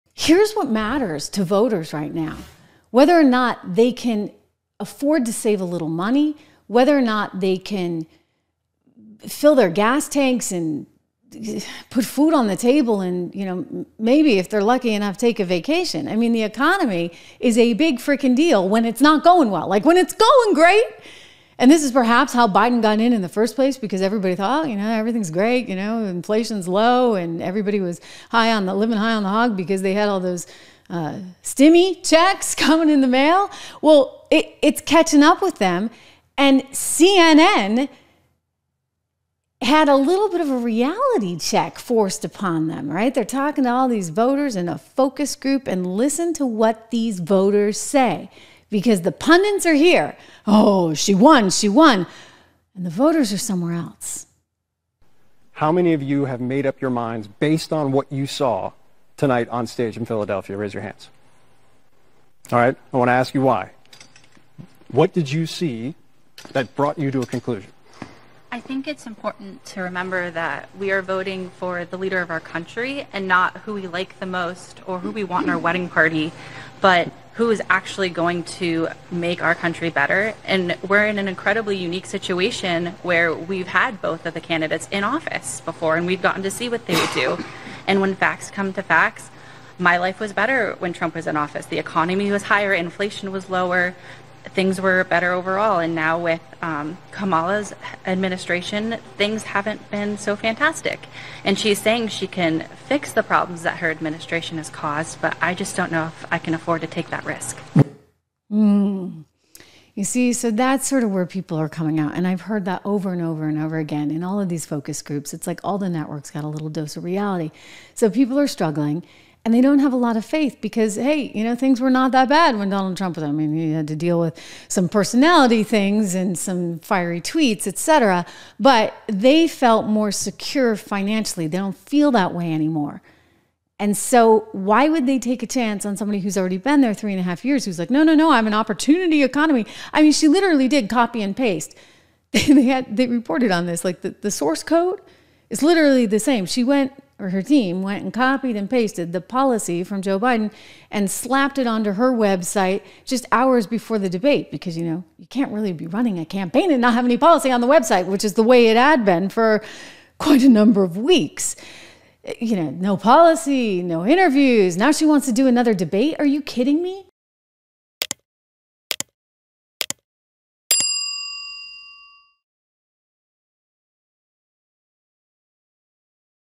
CNN Gets HARSH Reality Check From Voters After Debate!
These focus groups did not respond how CNN expected them to.